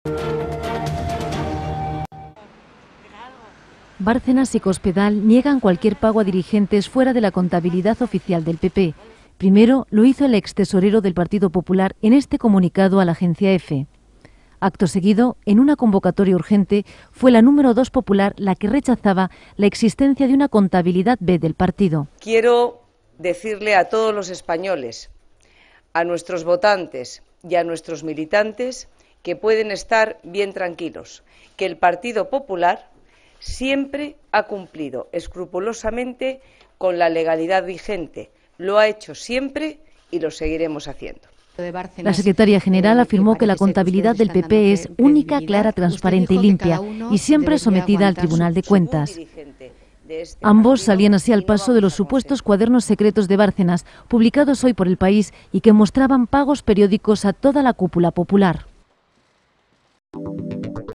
Cospedal afirma que "la contabilidad del PP es única, clara, transparente y limpia"
Madrid, 31 enero (CERES TV)

La secretaria general del PP, María Dolores de Cospedal, ha garantizado hoy que su partido ha cumplido siempre con la legalidad vigente, ha dicho que la contabilidad es "única, clara, transparente y limpia", y ha avanzado acciones legales para desmentir las informaciones publicadas.

Cospedal ha comparecido en rueda de prensa en la sede del PP para negar "rotundamente" la información del diario El País sobre una supuesta contabilidad manuscrita de los extesoreros del PP Álvaro Lapuerta y Luis Bárcenas que refleja pagos a la cúpula del partido, incluido el jefe del Ejecutivo, Mariano Rajoy, e ingresos por donaciones de empresas.